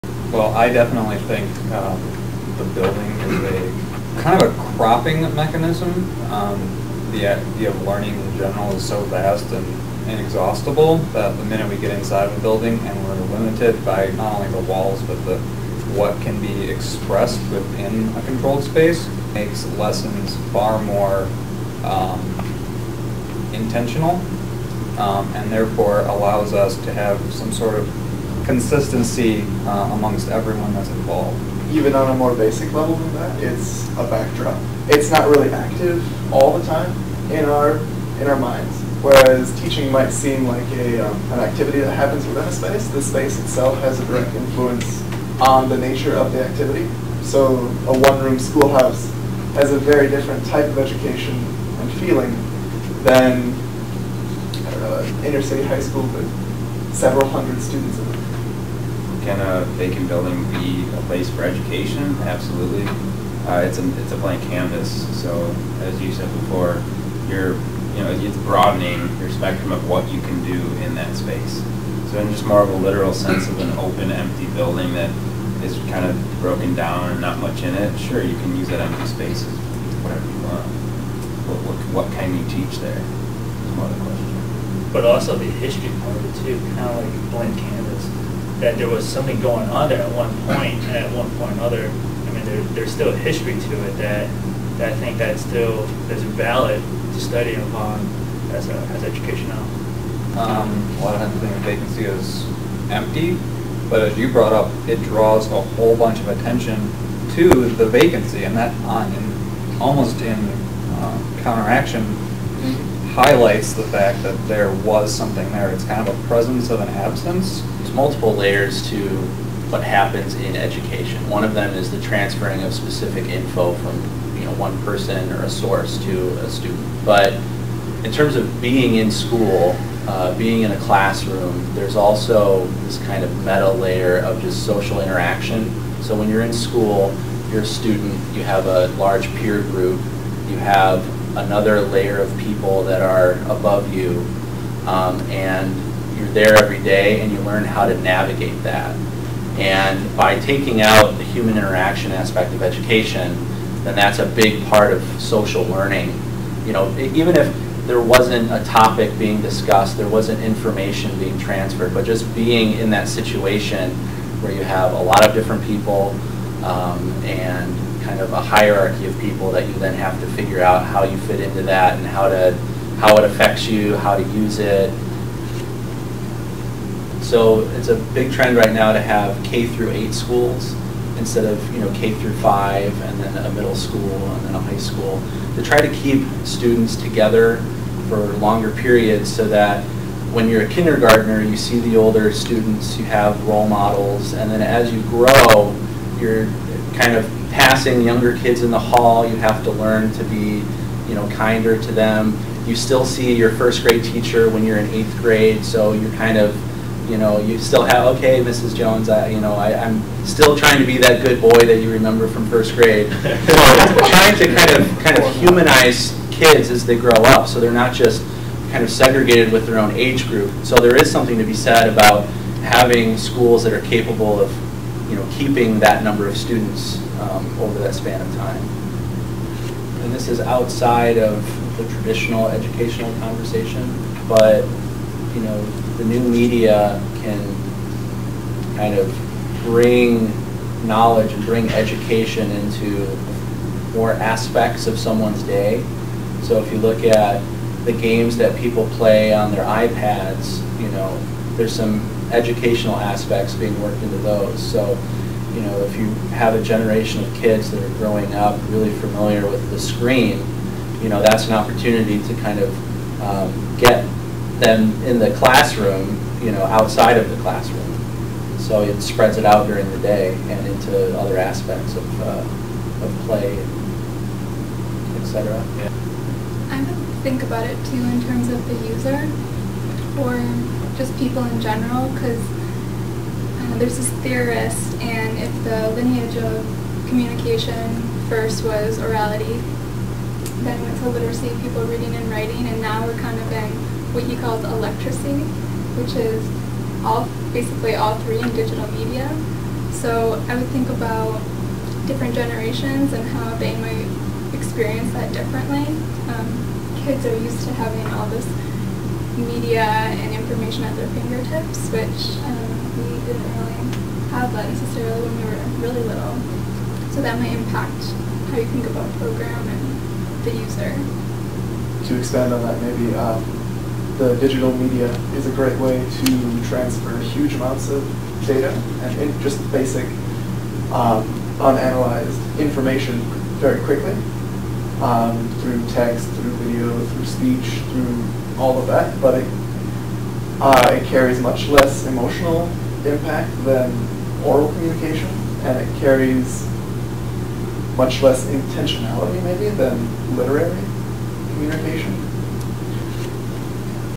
Project H.A.U.N.T. Media Prototype
This is a crude prototype of what might be projected onto abandoned buildings in Detroit for the project. I mixed pieces of audio from the interview with SmithGroupJJR together with videos showing moments of learning in different environments.